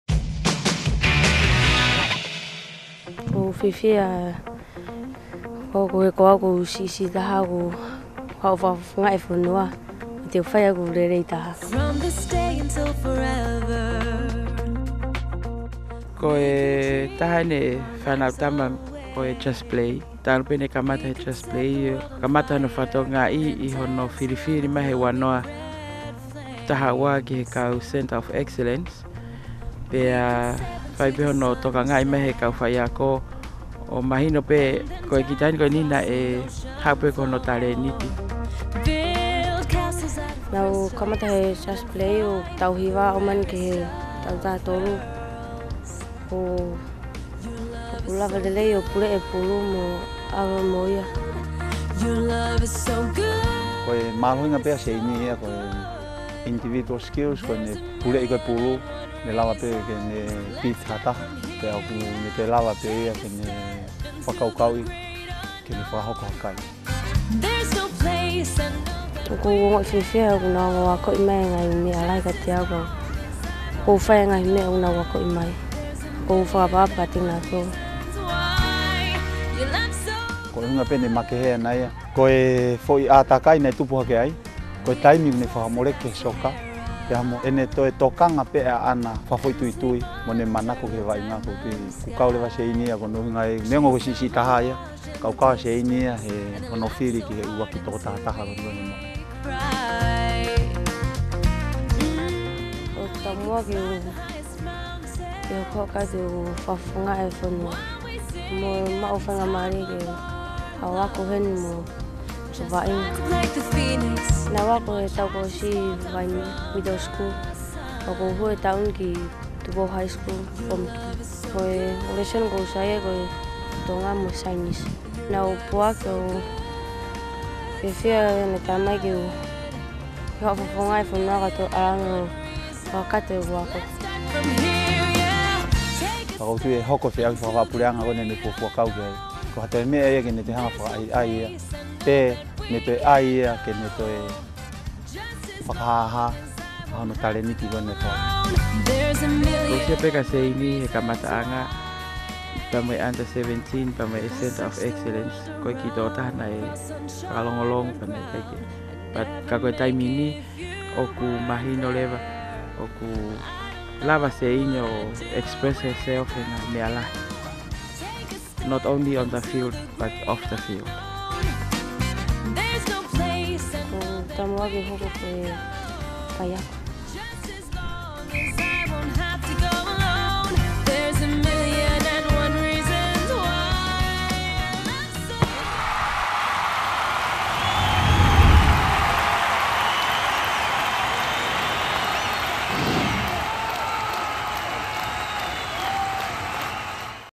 2014 OFC U-20 Women's Championship / Seini Lutu (Tonga)
OFC TV caught up with 13-year-old Seini Lutu, Tonga's #11, ahead of the OFC U-20 Women's Championship scheduled to be held at Mangere's Centre Park from 18-22 February 2014. Seini developed her love affair for the game through OFC's social development program Just Play.